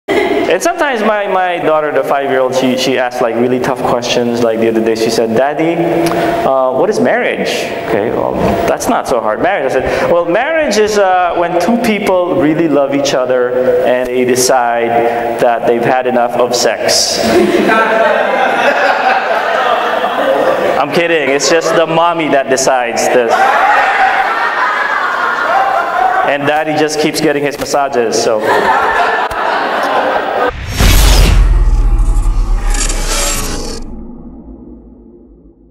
Standup Comedy: Husband Wife Jokes
My daughter asks me a question about what marriage is and of course I answer her with the truth about before and after marriage.